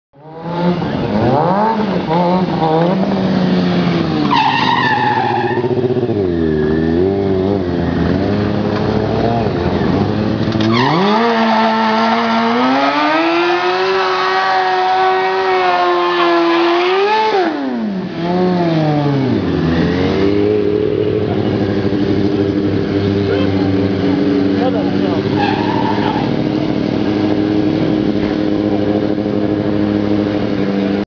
5th Gear Motorcycle Stunt Team Warm-ups
Stunt warm-ups at the 7th annual Vancouver Firefighter's Burn Ride in Harrison Hot Springs.